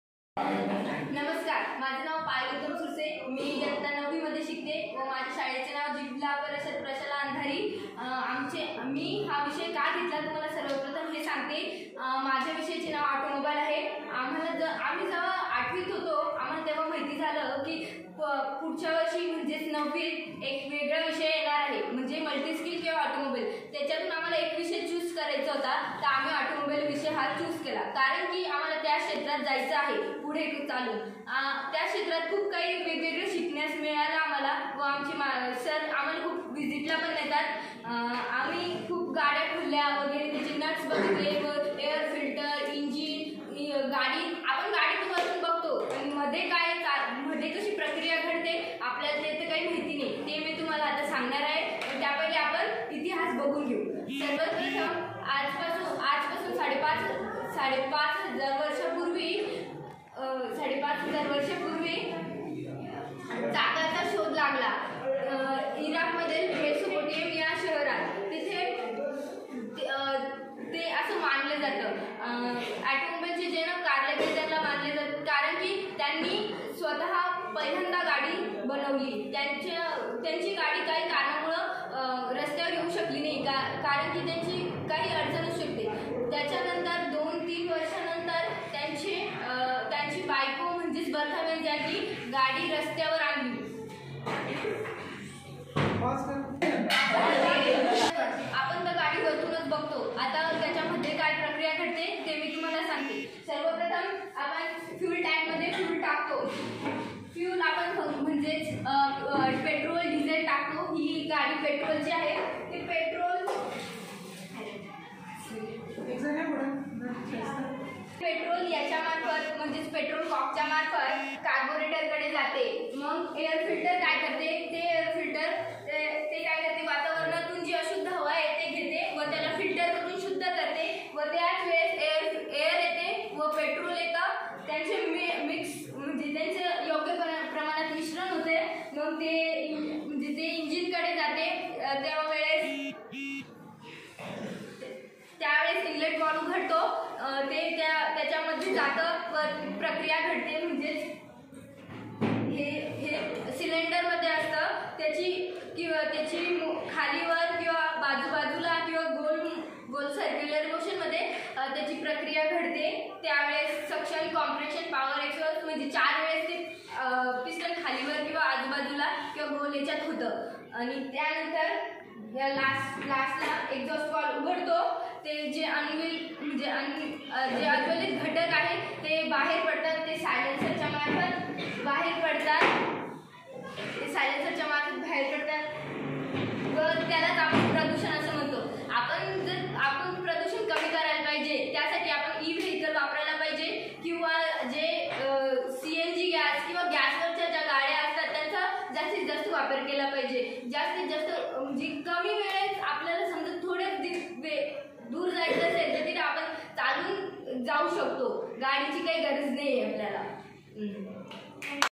प्रशाला अंधारी ऑटोमोबाईल ची ओळख- फ्युल सप्लाय सिस्टीम
जि. प.प्रशाला, अंधारी येथे सुरू असलेल्या व्यवसाय प्रशिक्षण अंतर्गत वर्ग 9 वी ची एक पायल नावाची ek विद्यार्थिनी... अत्यंत उत्स्फूर्तपणे ऑटोमोबाईल विषयी दिलेली माहिती..!!!
आभार:- मुख्याध्यापक श्री एकनाथ जाधव
        व्यवसाय प्रशिक्षक :- धीरज दिलीपकुमार कासलीवाल